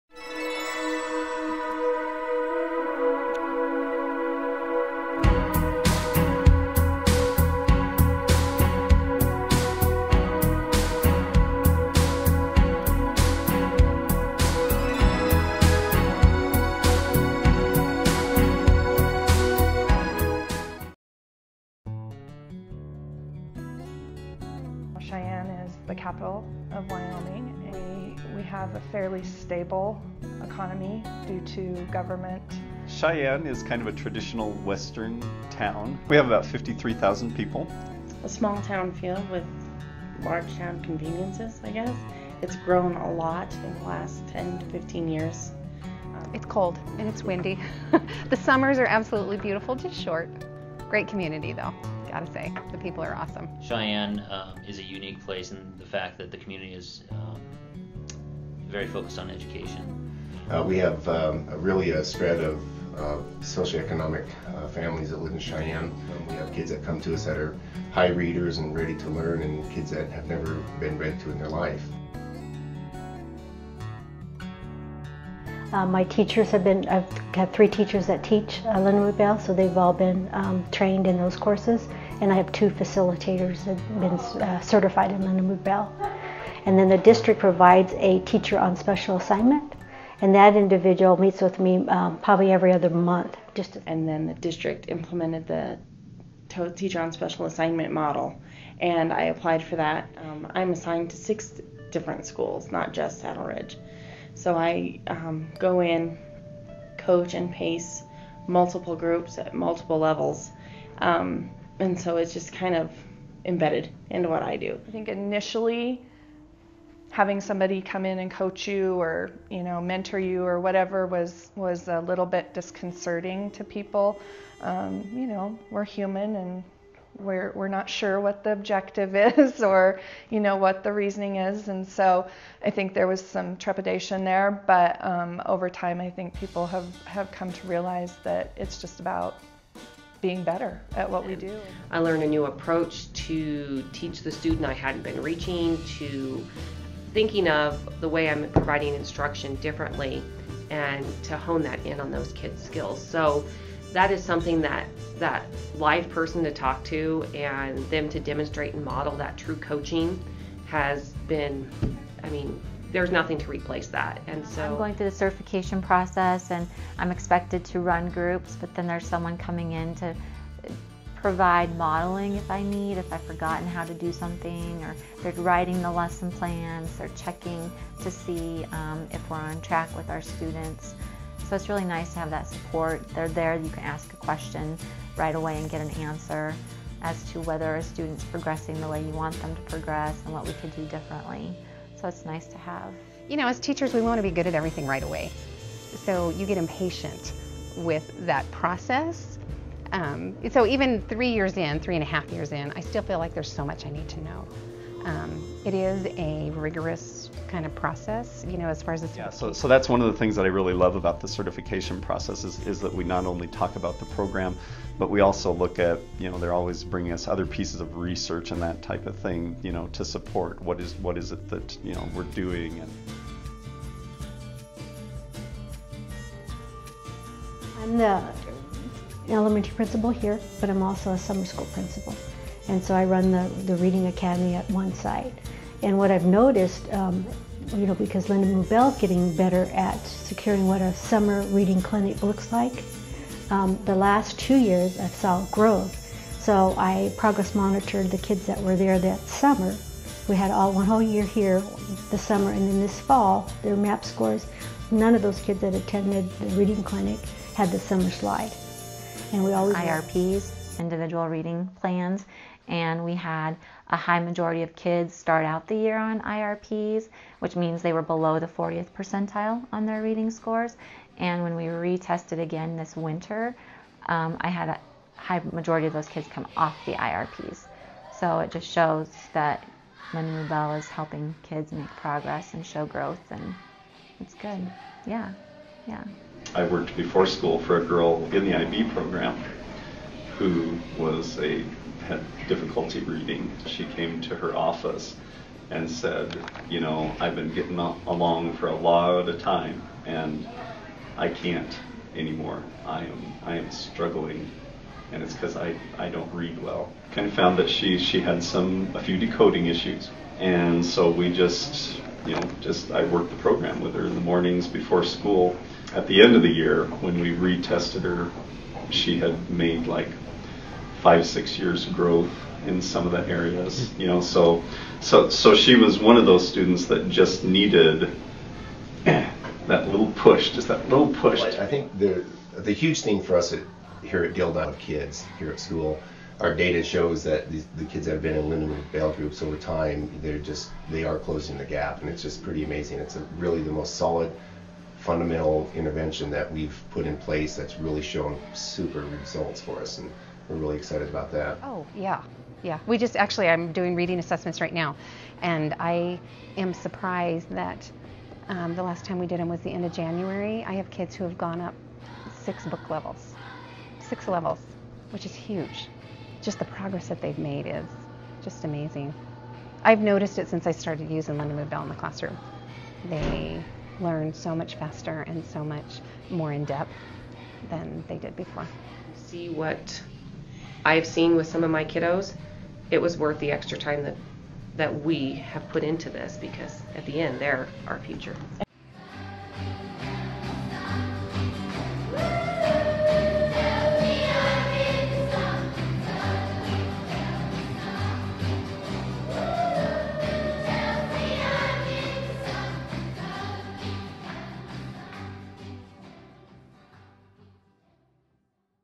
Laramie County School District 1 Cheyenne, Wyoming
Closing the Achievement Gap with a Lindamood-Bell School Partnership

Job-embedded professional development in research-validated instruction has improved teacher effectiveness, and has raised student reading scores dramatically. In addition, students' improved  reading and comprehension is key to their ability to access the Common Core.
Watch the video to get the full story--